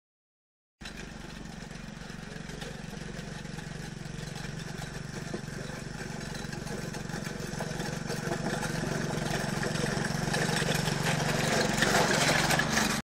Испытания культиватора в зимнем варианте №1. Мини вездеход.
Испытания прошли не очень удачно, по окончанию заезда отвалилось колесо.